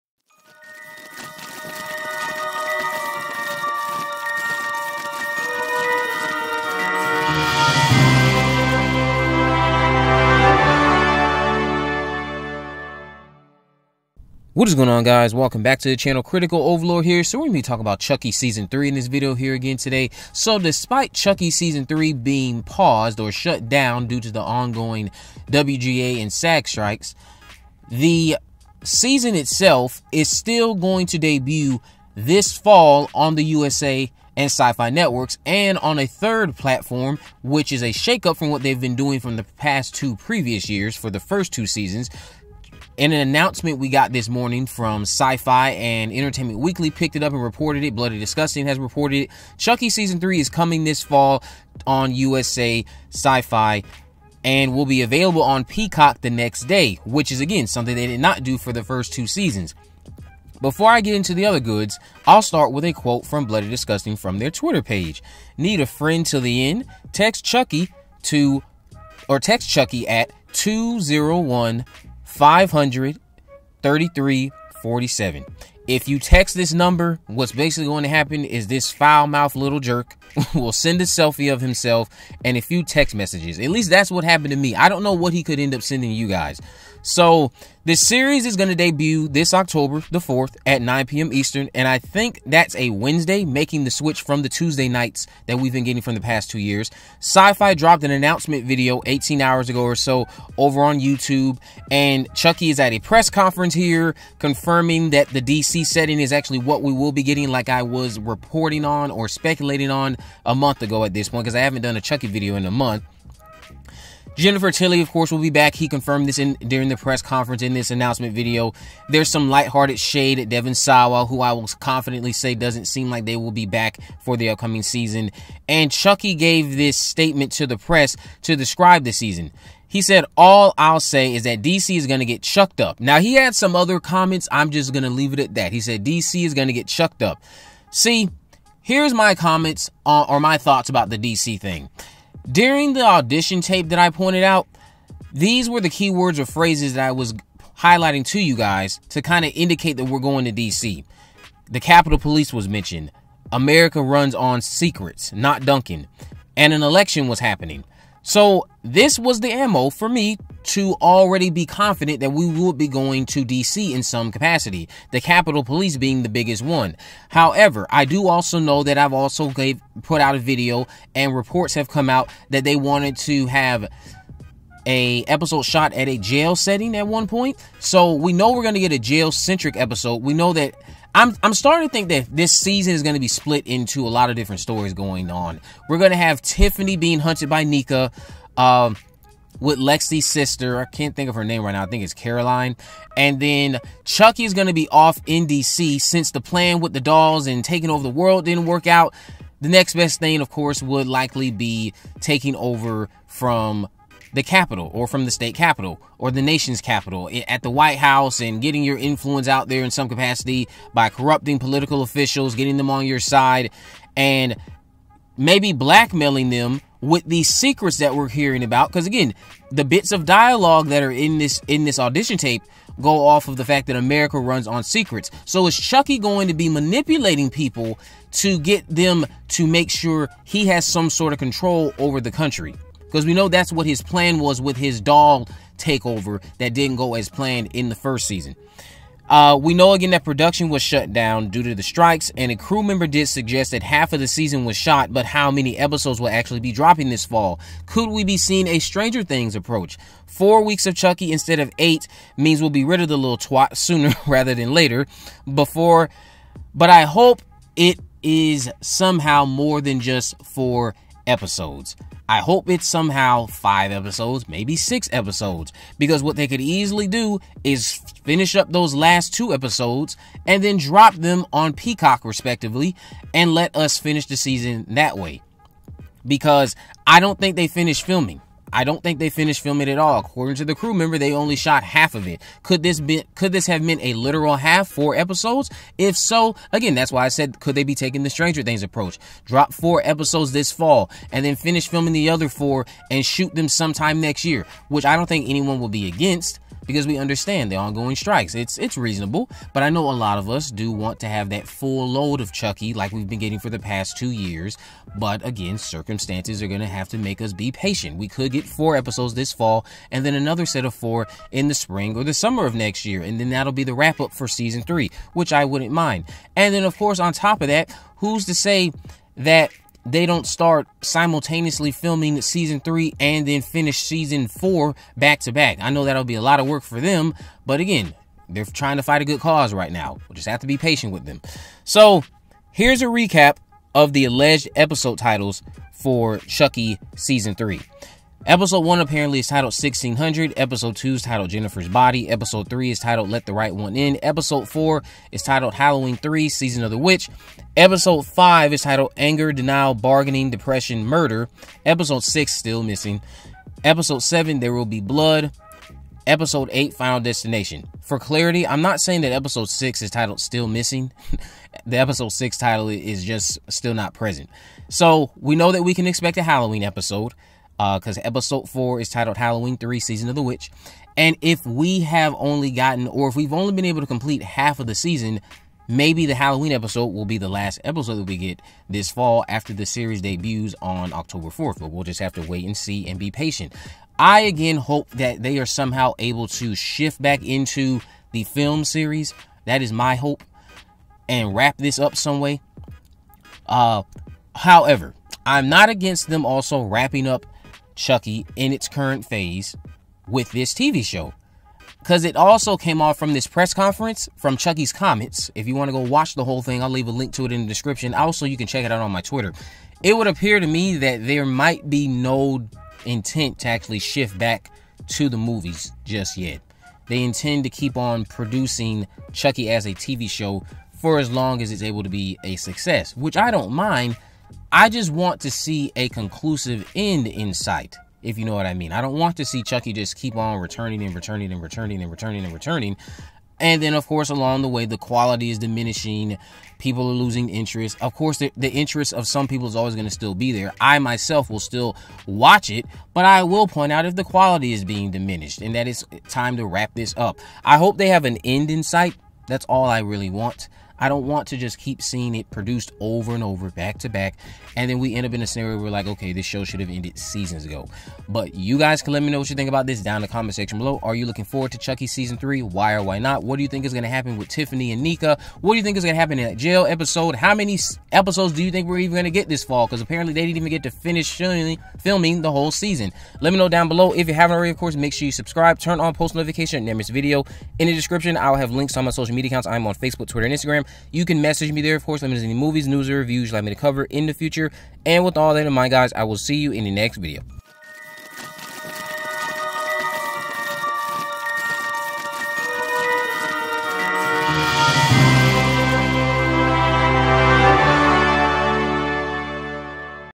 CHUCKY | Season 3 ARRIVES This October + Good Guys Coming To D.C. Confirmed
Chucky Season 3 is coming to USA/Syfy according to a recent report from Don Mancini and Deadline. Chucky Season 3 will debut later this Fall it seems and promises to be the scariest season ever. Chucky Season 2 ended this past year, but fans were waiting for the official announcement of Season 3. Recently, the alleged official titles for Chucky's upcoming season have been revealed by spoilertv. Production has paused on Season 3, but new audition tapes have been discovered. Good Guys might be headed to DC for some chaos. EW has confirmed Chucky Season 3 arrives this October and the story is headed to D.C.

Audition Tapes -